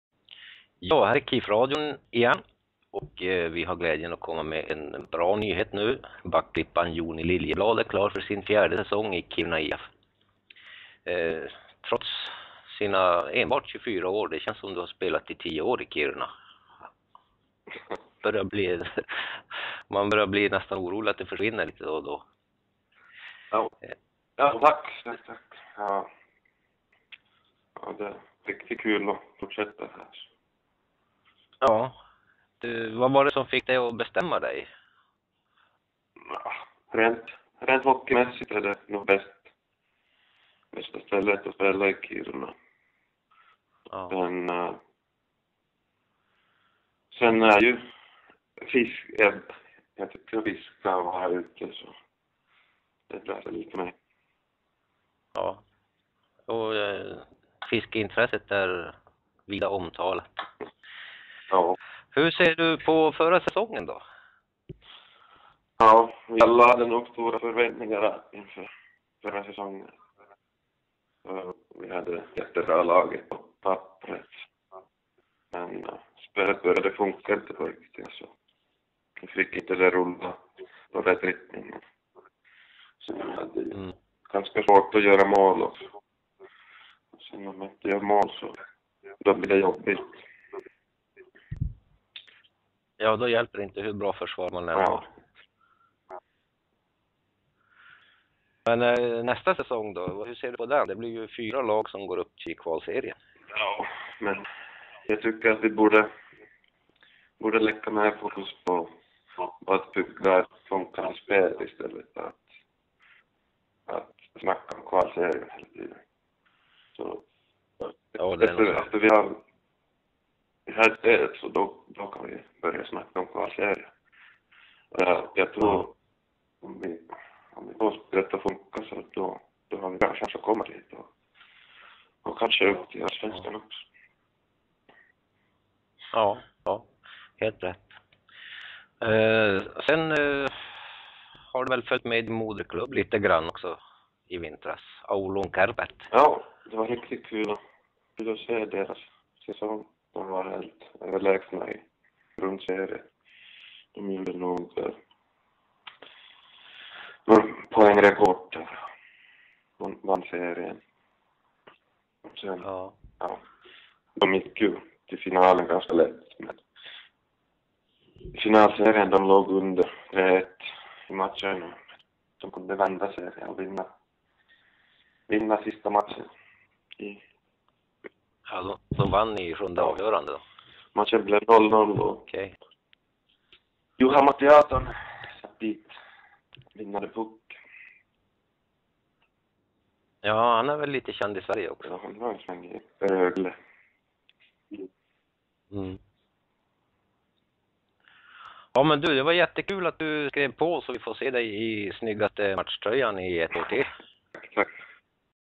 KIF Radio - Joni Liljeblad
Liljeblad klar för sin fjärde säsong i gruvstaden.
"Hockeymässigt är Kiruna det bästa alternativet"